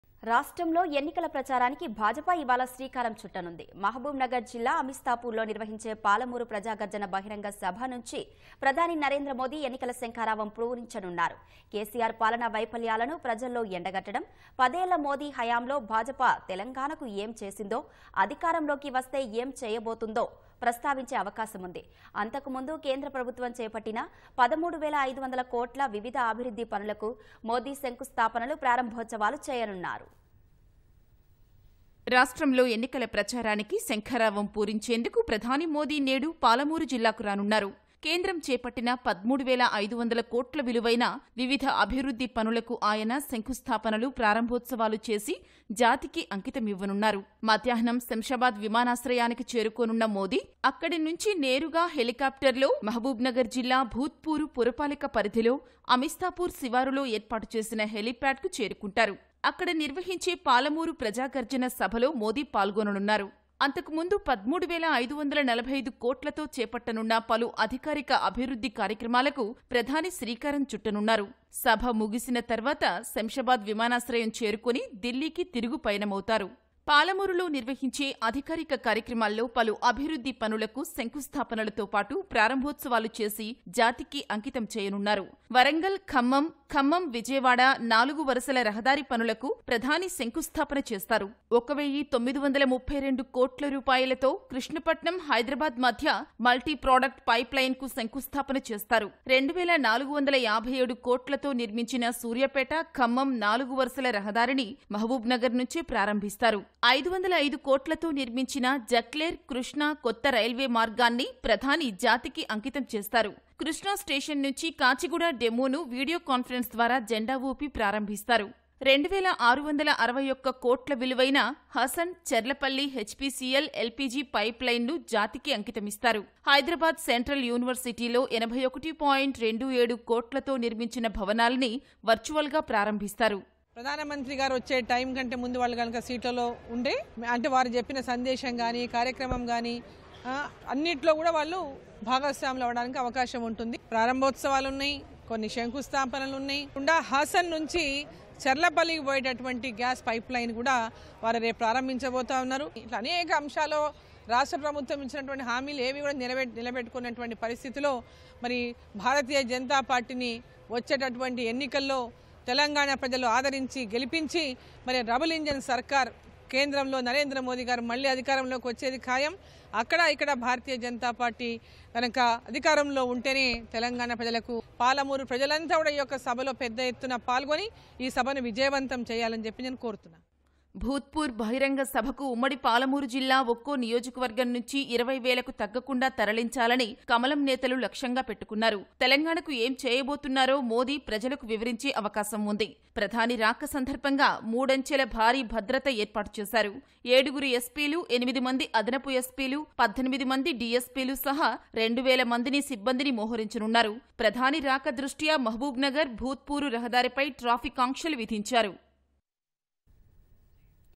BJP Palamuru Praja Garjana Sabha Public Meet | PM Modi Attend | ప్రజాగర్జన సభతో భాజపా ఎన్నికల భేరి
రాష్ట్రంలో ఎన్నికల ప్రచారానికి భాజపా ఇవాళ శ్రీకారం చుట్టనుంది. మహబూబ్ నగర్ జిల్లా అమిస్తాపూర్ లో నిర్వహించే పాలమూరు ప్రజాగర్జన బహిరంగ సభ నుంచి ప్రధాని నరేంద్ర మోదీ ఎన్నికల శంఖారావం పూరించనున్నారు. KCR పాలనా వైఫల్యాలను ప్రజల్లో ఎండగట్టడం, పదేళ్ల మోదీ హయాంలో భాజపా తెలంగాణకు ఏం చేసిందో, అధికారంలోకి వస్తే  ఏం చేయబోతోందో ప్రస్తావించే అవకాశం ఉంది. అంతకుముందు కేంద్ర ప్రభుత్వం చేపట్టిన 13వేల 500 కోట్ల వివిధ అభివృద్ధి పనులకు మోదీ శంకుస్థాపనలు, ప్రారంభోత్సవాలు చేయనున్నారు.